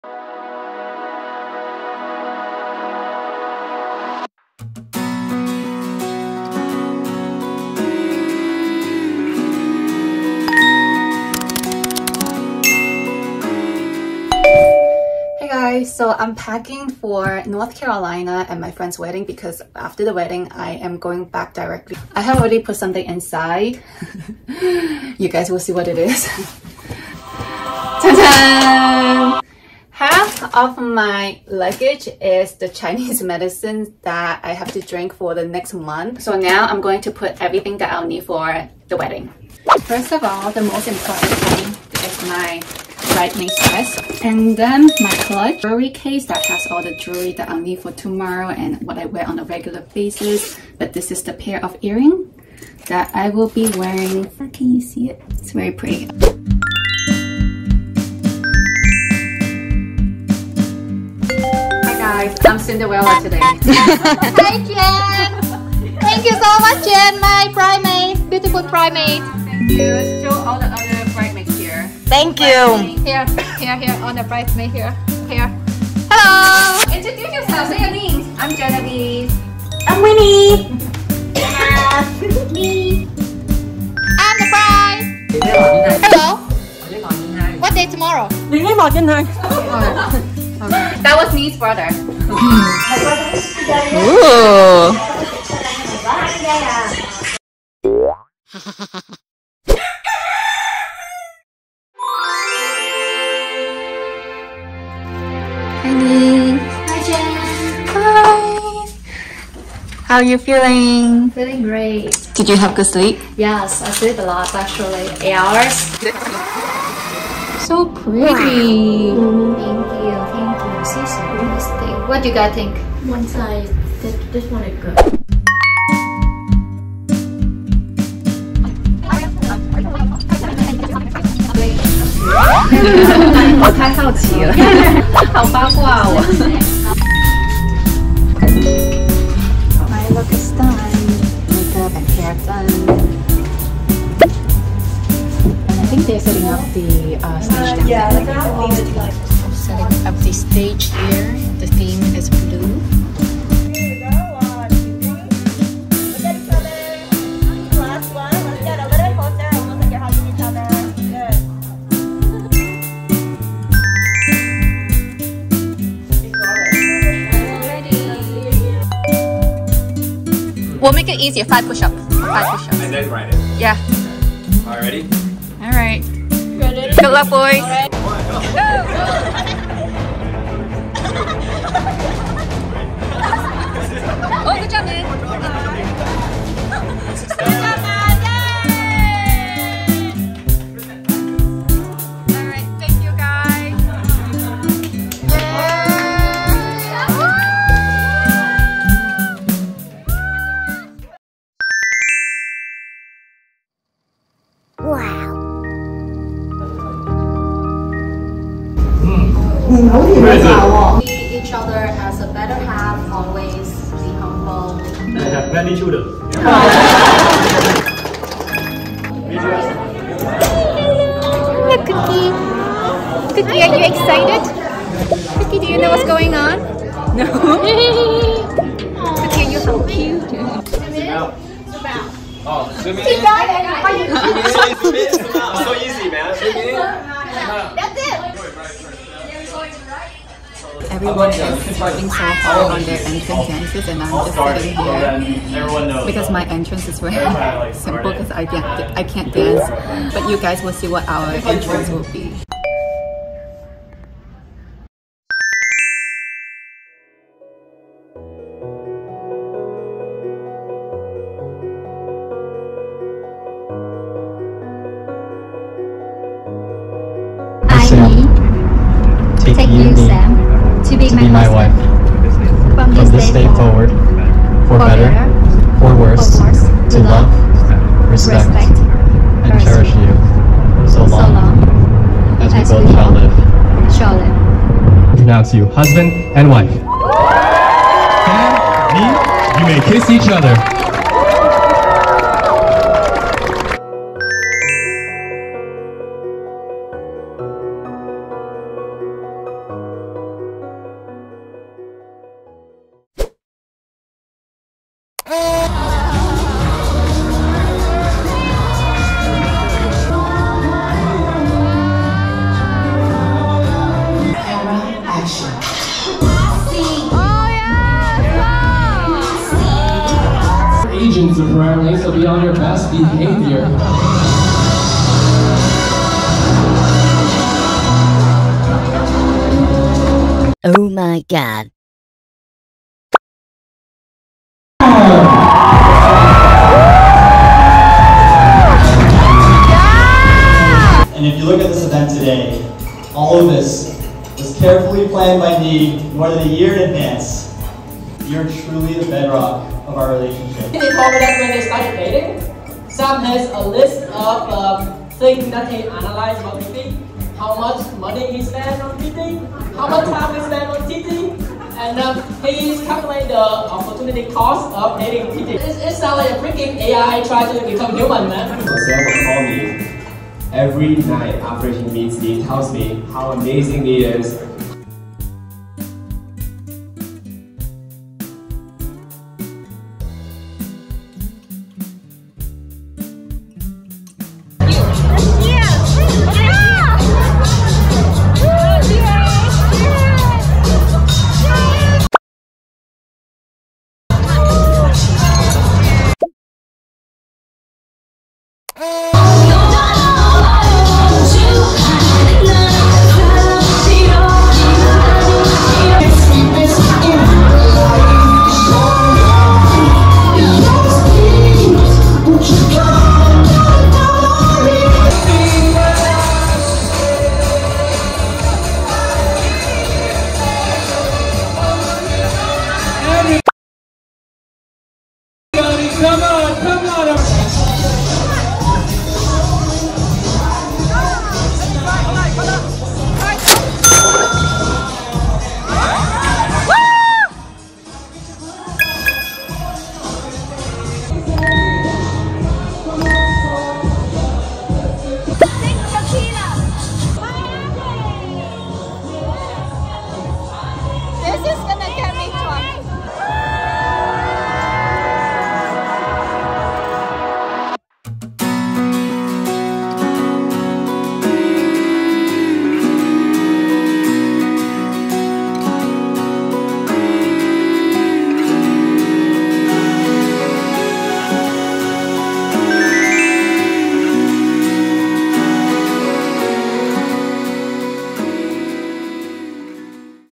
Day in my life📝 | First Time as Bridesmaid in NYC in a Dreamy Wedding 纽约伴娘初体验 梦幻花海婚礼 [吃喝玩乐Vlog]
Hi everyone! (•◡•) /

This week's vlog is a very special one- I am a bridesmaid at my dear friend Nhi's wedding! This was unquestionably the most sophisticated and dreamy wedding I have been to, Nhi is raising the bar for all of us! 😂

This remarkable wedding was filled with love, grace, and charm. I am glad that I was able to capture bits of this special moment for our lovely sunshine bride ❤️

I hope that you find this vlog enjoyable!

大家Hello 欢迎收看新一期的吃喝玩乐Vlog :)

我这次做伴娘咯！这是我在纽约的第一次伴娘体验，很特别。 新娘是我的好朋友，很sweet很阳光的一位女生。很窝心可以陪着她出嫁。这次的vlog记录了我的伴娘过程，希望大家喜欢这期的vlog吧！

Cheers,
(っ＾▿＾)۶🍸🌟🍺٩(˘◡˘ )

What I Wore:
Earrings

Jewelry Case

Timecodes
0:00 - Intro
0:14 - About this Vlog | Packing List for the wedding
0:55 - Final Preparations in the Bride's room and Introductoions
1:45 - Wedding Day! Getting makeup and hair done
2:51 - Game time with the groom and groomsmen
3:21 - Tea Ceremony tradition
3:52 - Bridesmaid & Groomsmen practicing entrance dances
4:15 - Official Wedding Ceremony
5:06 - Dinner entrances & Dinner Speeches  & Dinner!
6:36 - Surprise Dance prepared by the Bride for the Groom and guests
7:35 - Closure & Photos

Editing Software:
Davinci Resolve 18 Studio